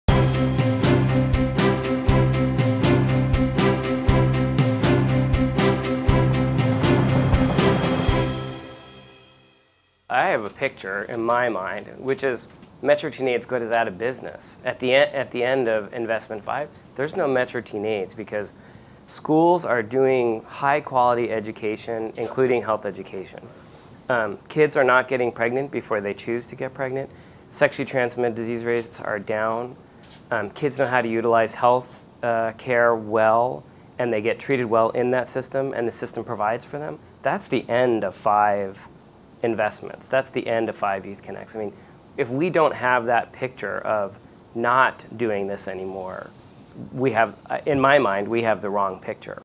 Success for youthCONNECT
Adam Tenner of Metro TeenAIDS discusses what a successful collaborative initiative looks like for his organization: Going out of business